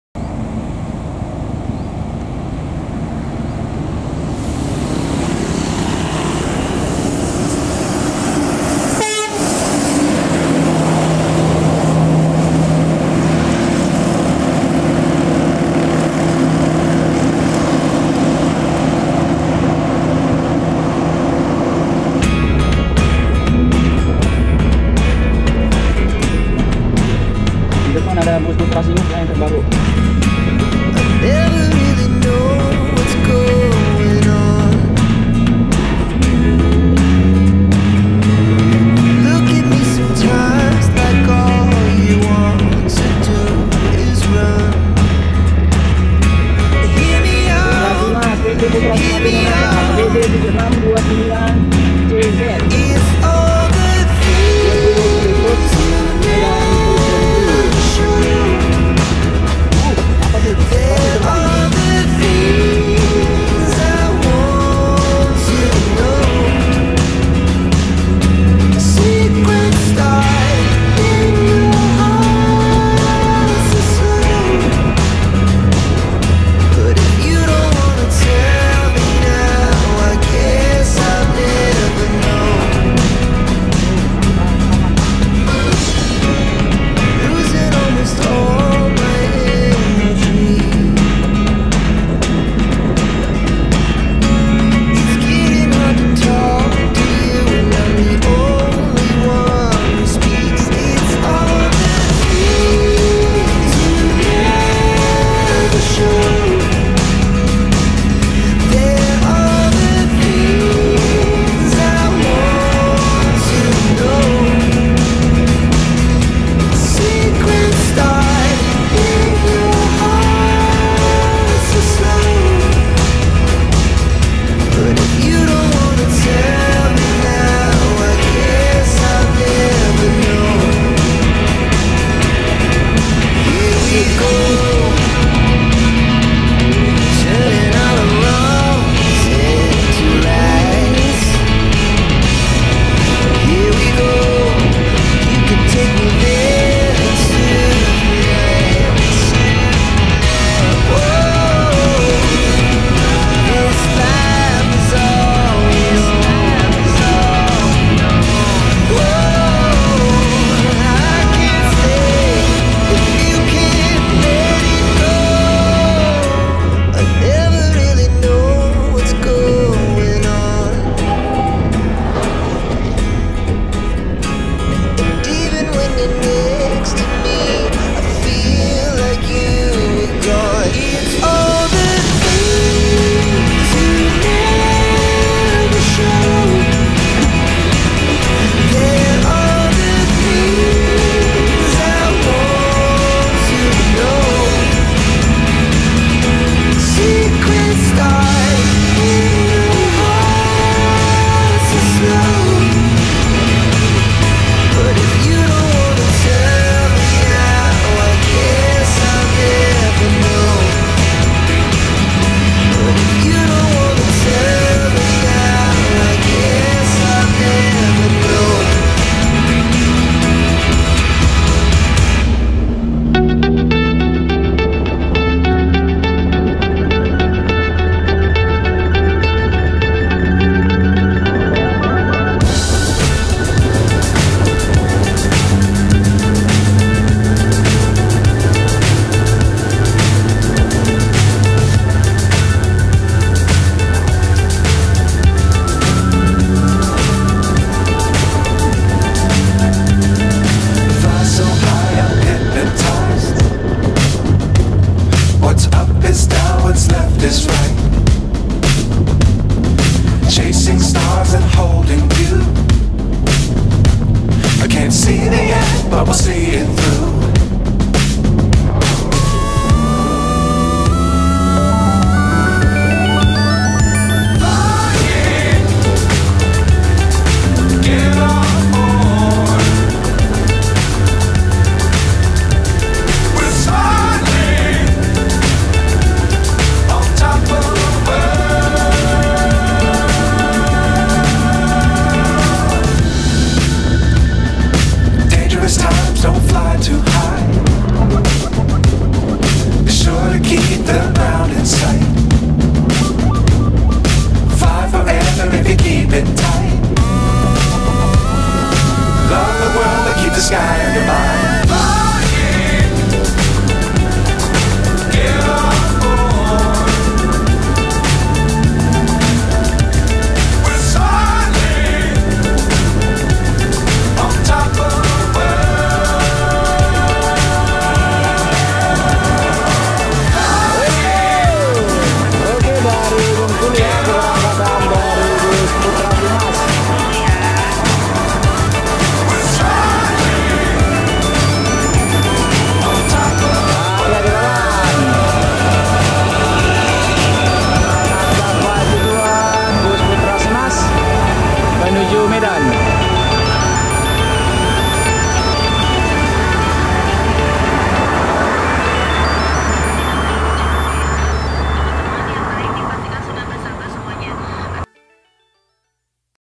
Perjalanan Bus putra simas bengkulu medan sebelum masuk tol Pekanbaru
Bus putra simas lewat, langsung kita ikuti perjalanan sebelum masuk tol Pekanbaru Dumai.